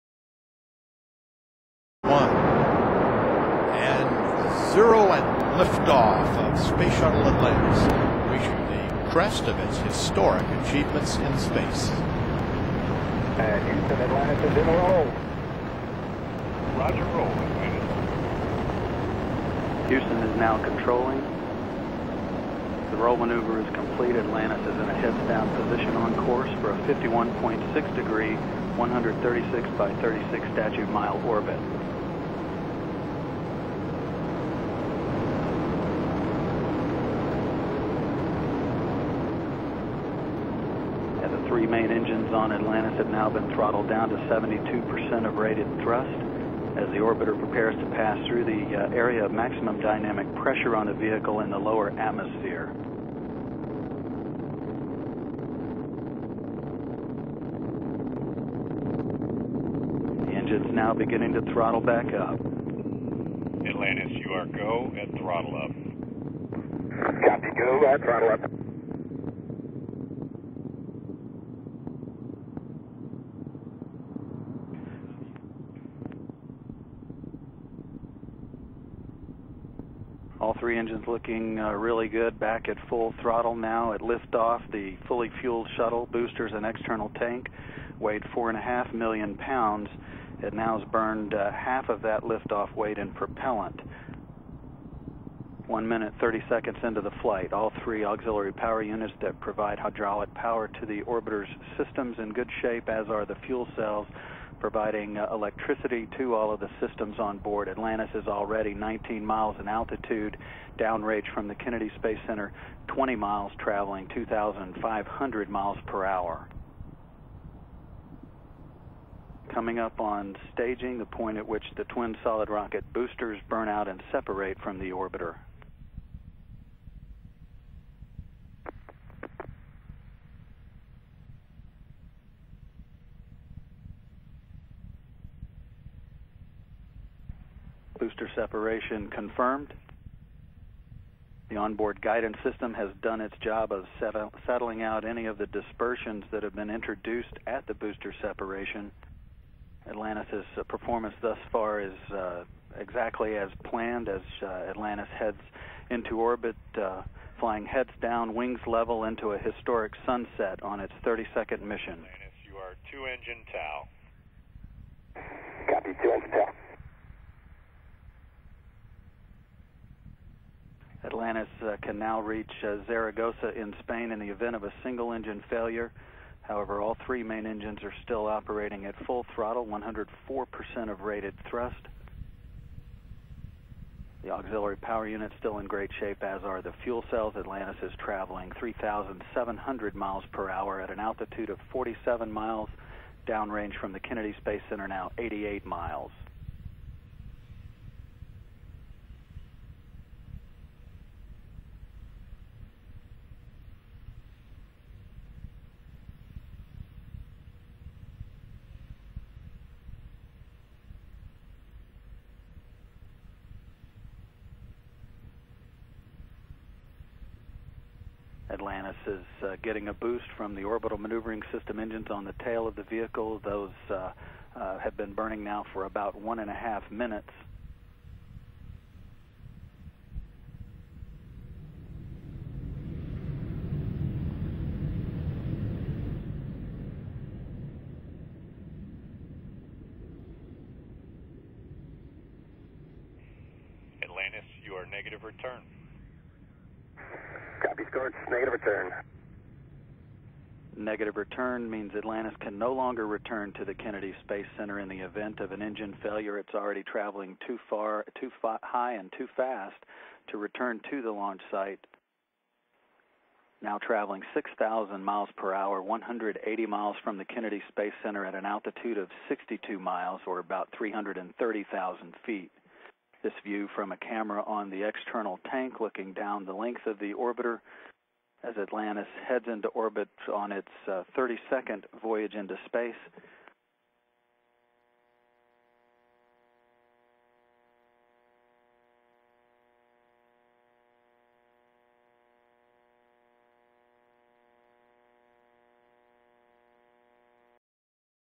STS-132: Atlantis Launch
May 14,2010
STS-132: Space Shuttle Atlantis Launch